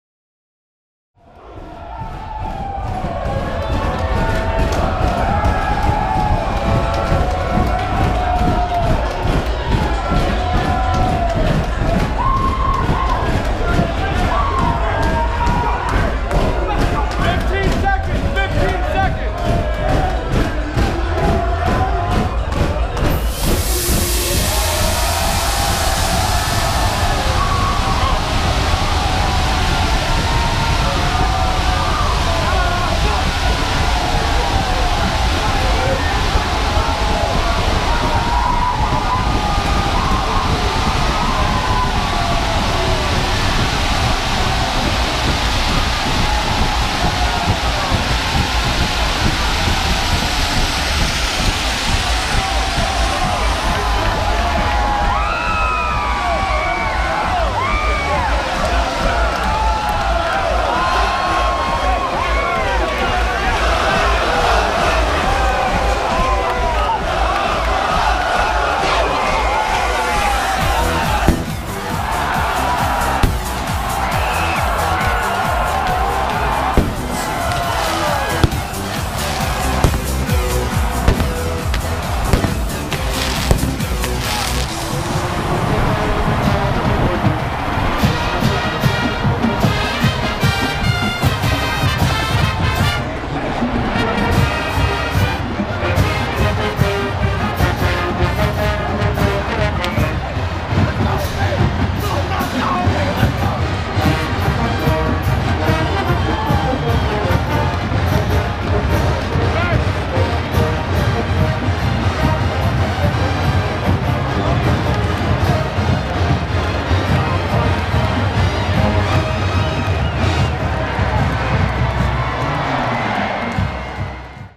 Run Out with the NC State Wolfpack (360 video)
Hear the roar of Wolfpack Nation as they take the field. Fans can now get a glimpse of the experience through this immersive 360 video (Once the video starts, click and drag to look around.)

You can also load the video on a Google Cardboard and feel like part of the Wolfpack!